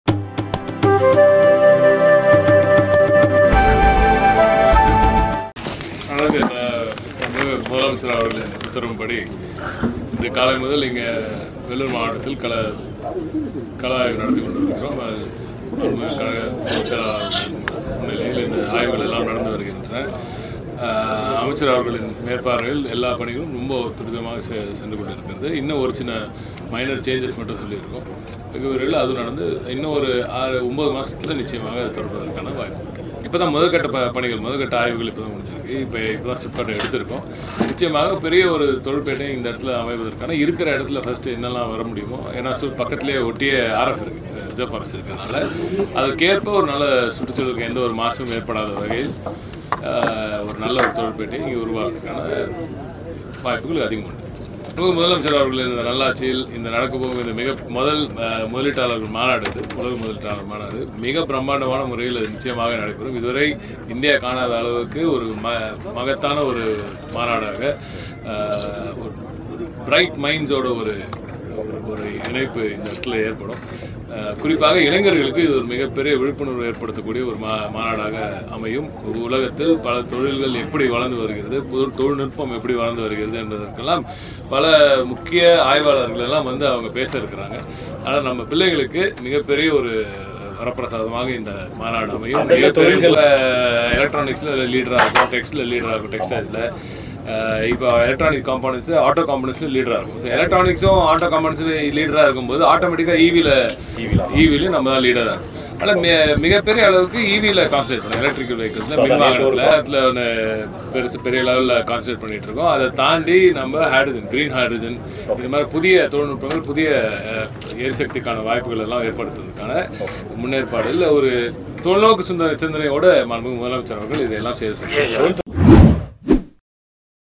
முதல்வரின் தொலைநோக்கு சிந்தனை | TRB ராஜா பெருமிதம்! | TRB Raja Pressmeet
சாணக்யா!